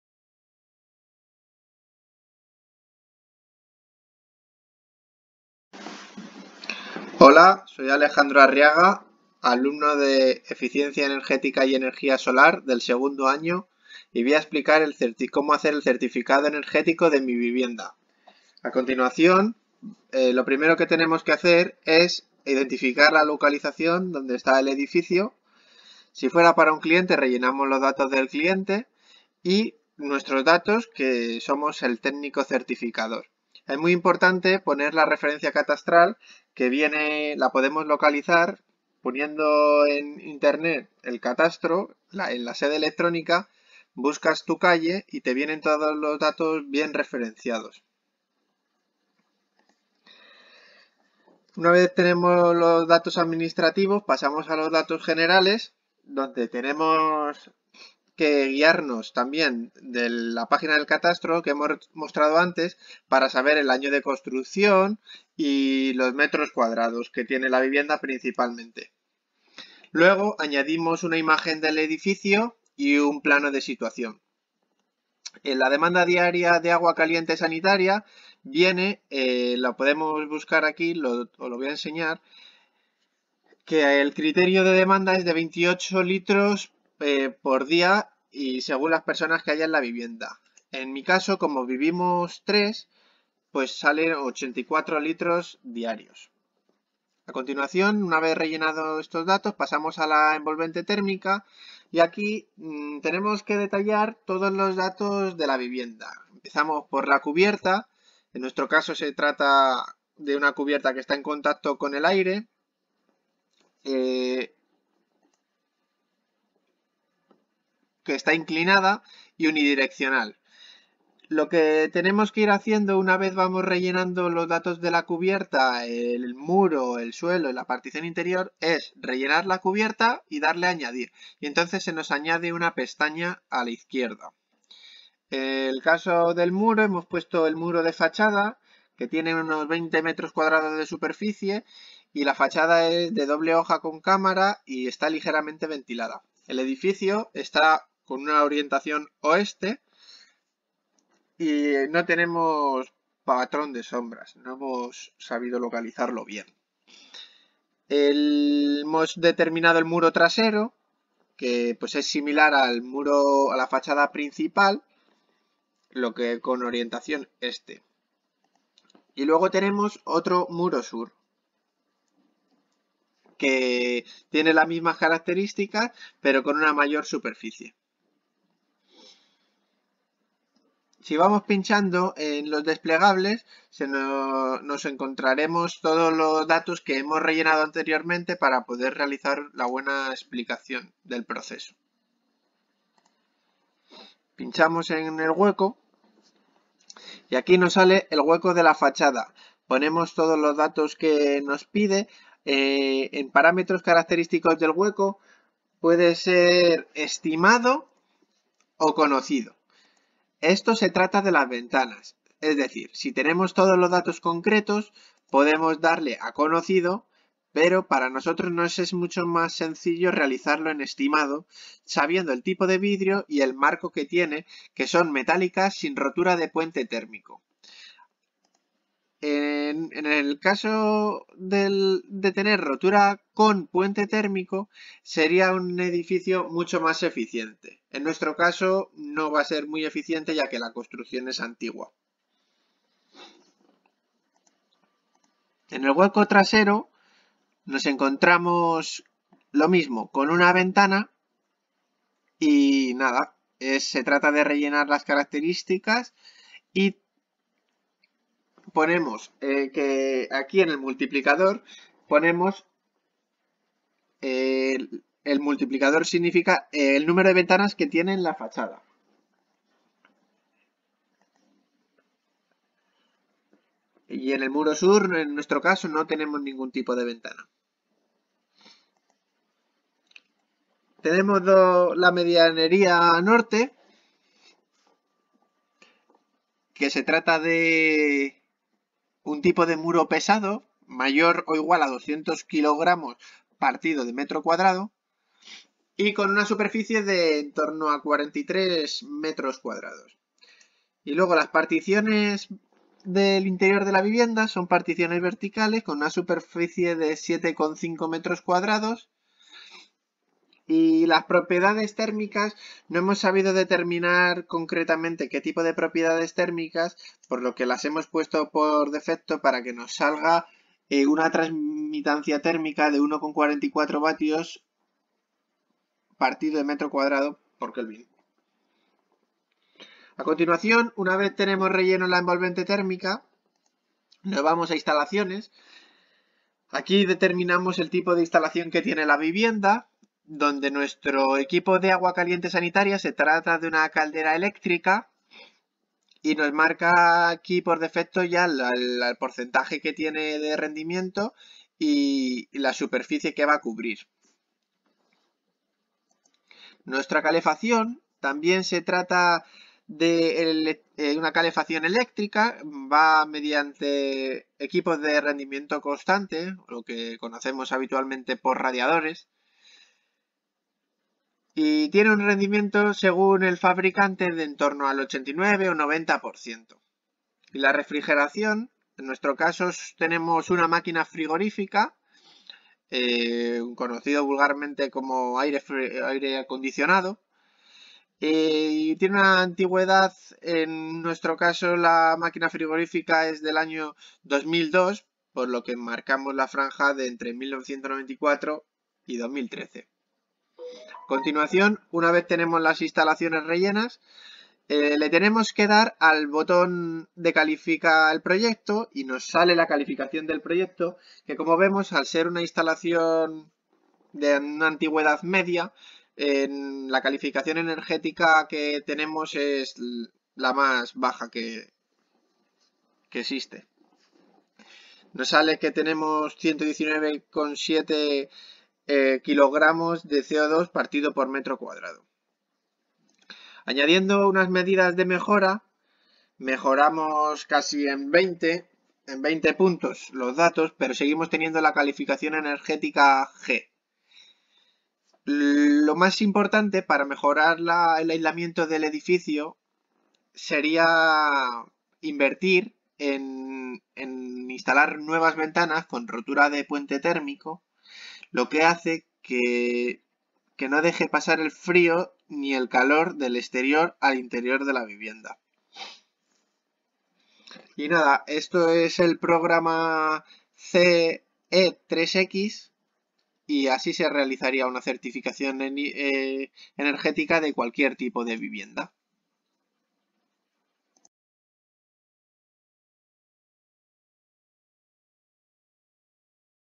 Certificado de eficiencia energética residencial en Madrid (8)
Alejandro Arriaga, estudiante de 2º curso del Ciclo formativo de grado superior de Eficiencia energética y energía solar térmica de Salesianos Carabanchel, construye un certificado de eficiencia residencial para una vivienda en Madrid. El contenido es el habitual en este tipo de certificados: datos administrativos, datos generales, parametrización de la envolvente térmica, modelización de instalaciones energéticas, calificación energética final y medidas de mejora. De forma muy correcta y didáctica, Alejandro va explicando el proceder habitual en este tipo de certificados. La calificación obtenida es muy modesta, incluso a pesar de las antedichas medidas de mejora propuestas. Agradecemos el buen hacer de nuestro alumno que confiamos os sea de utilidad a todos. Un fuerte abrazo.@FP Eficiencia energética y energía solar térmica